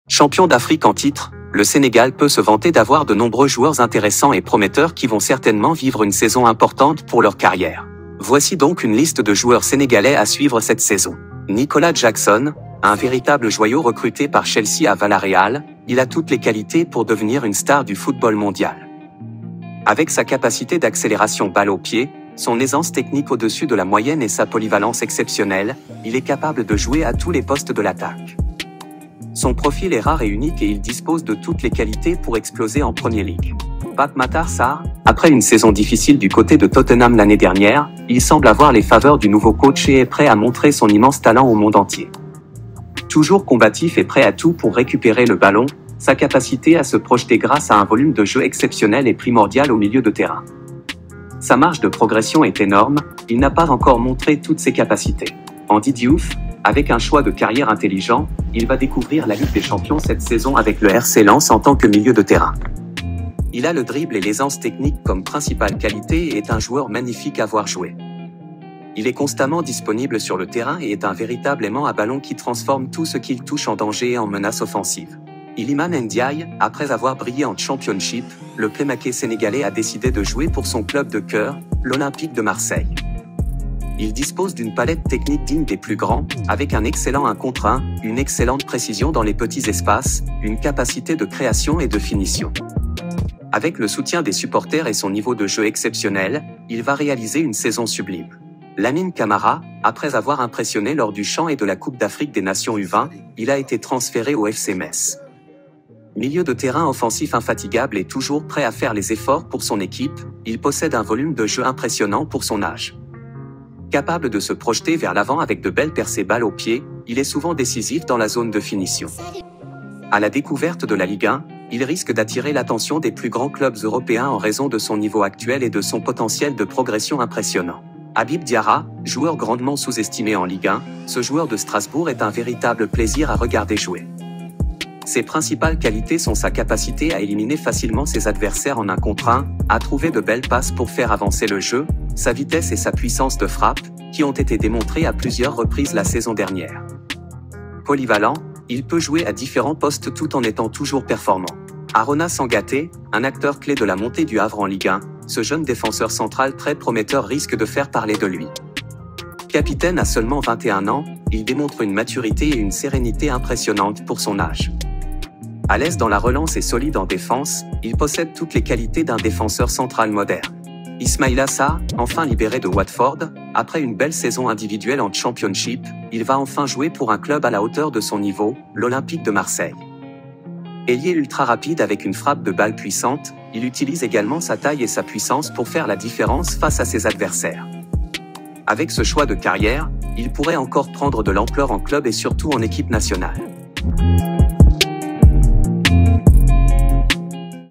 Les footballeurs sénégalais à suivre cette saison
Champion d'Afrique en titre, le Sénégal peut se vanter d'avoir de nombreux joueurs intéressants et prometteurs qui vont certainement vivre une saison importante pour leur carrière.

Voici donc une liste de joueurs sénégalais à suivre cette saison

Merci de liker la vidéo et de nous laisser votre avis en commentaire.